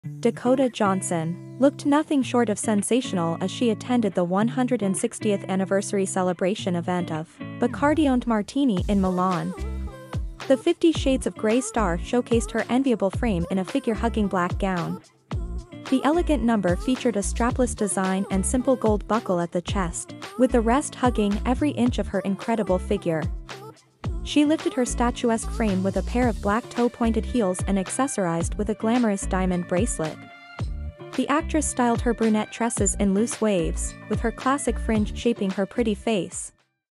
Dakota Johnson showcases her enviable frame in a figure-hugging black gown🖤
Dakota Johnson looked nothing short of sensational as she attended the 160th anniversary celebration event of Bacardi-owned Martini in Milan. The 50 Shades Of Grey star showcased her enviable frame in a figure-hugging black gown.
The elegant number featured a strapless design and simple gold buckle at the chest, with the rest hugging every inch of her incredible figure. She lifted her statuesque frame with a pair of black toe pointed heels and accessorised with a glamorous diamond bracelet.
The actress styled her brunette tresses in loose waves, with her classic fringe shaping her pretty face.